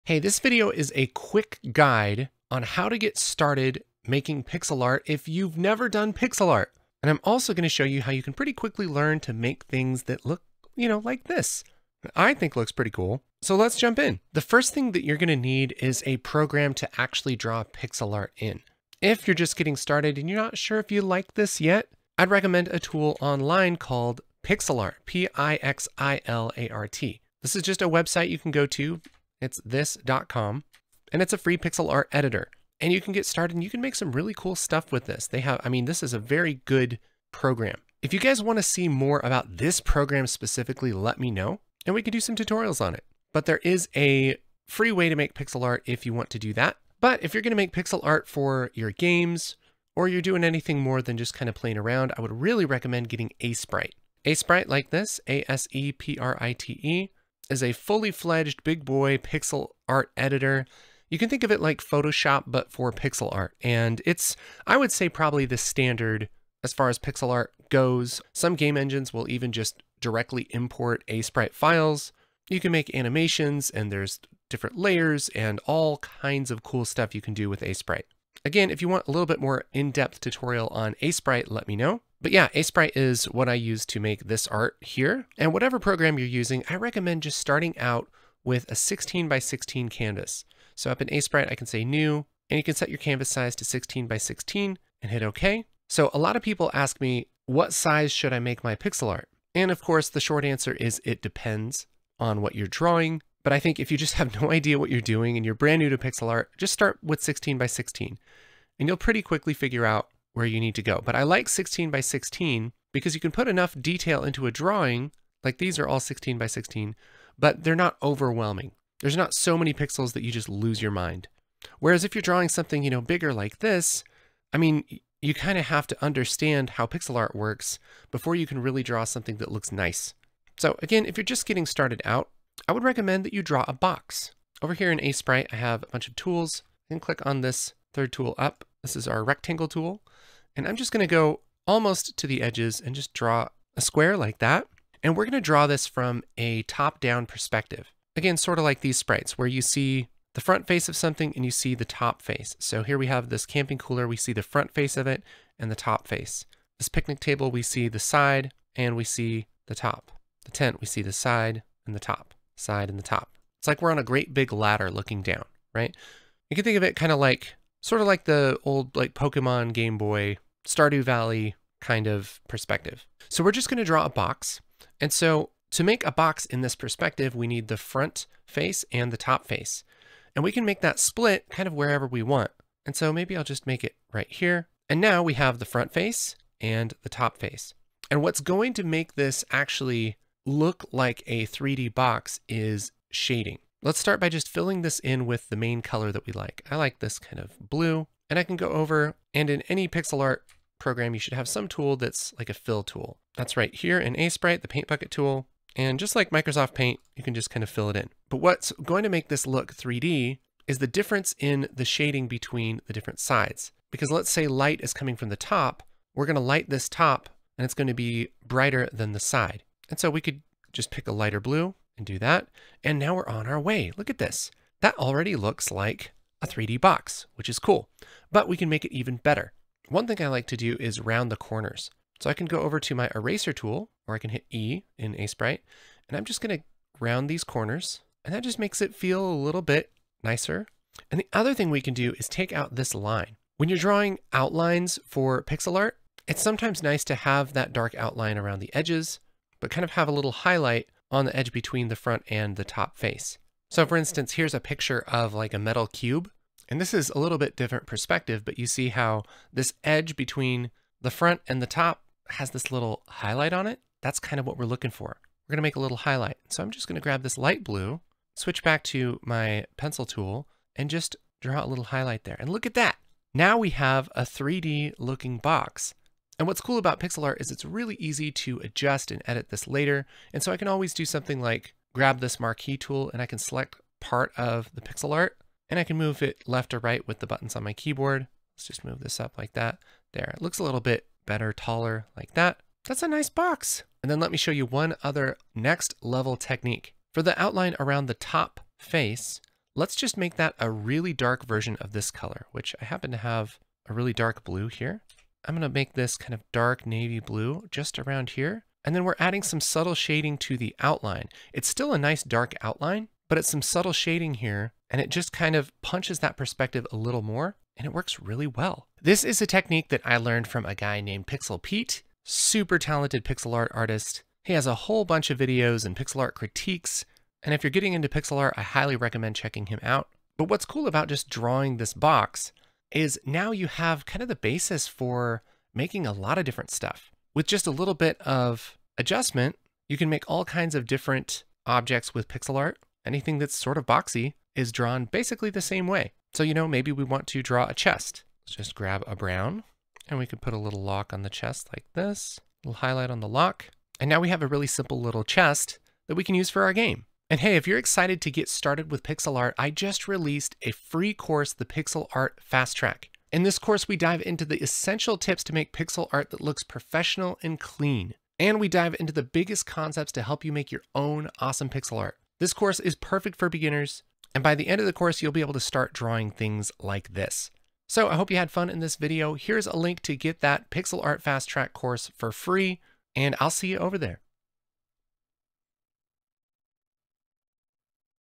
Pixel Art for Beginners - Getting Started
Here's how you can get started with pixel art EVEN if you know nothing about art!